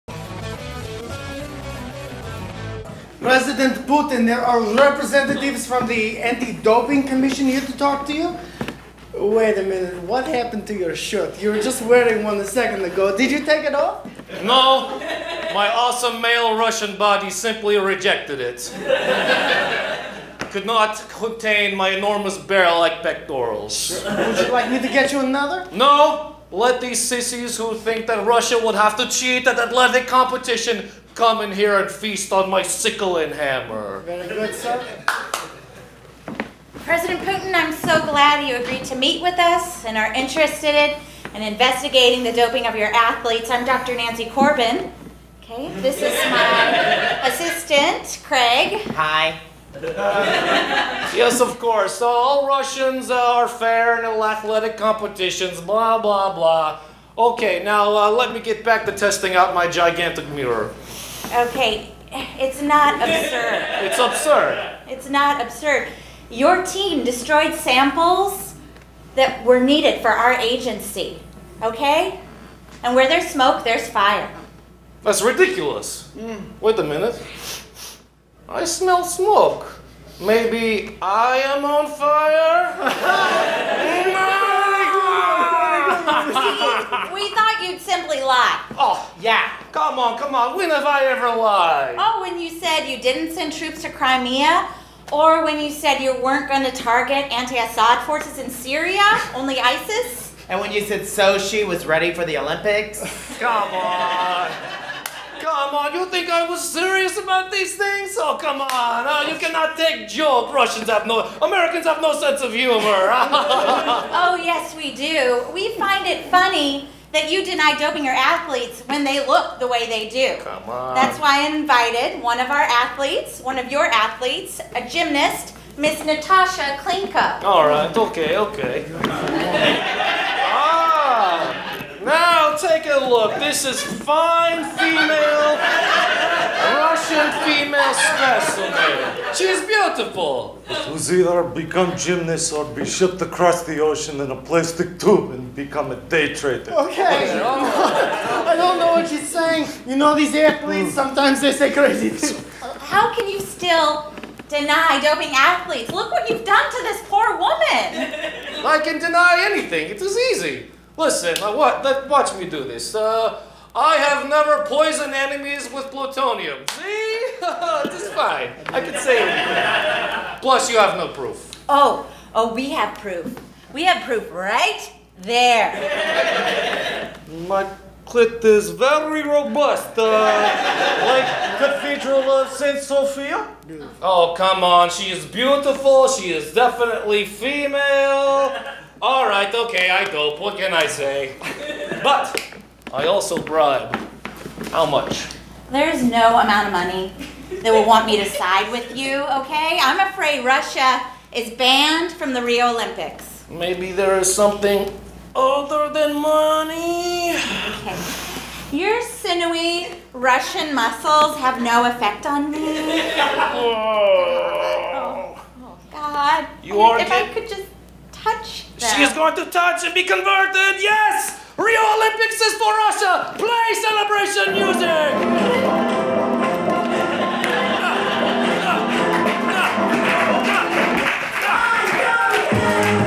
Russians Deny Doping Their Athletes (sketch)
Russian President Vladimir Putin loses his cool and his shirt over World Anti-Doping allegations!

Writer
Greg Shouse

Cast
Brian Vestal
Mike Falzone
Molly Fite
Phillip Wilburn
Nate Duran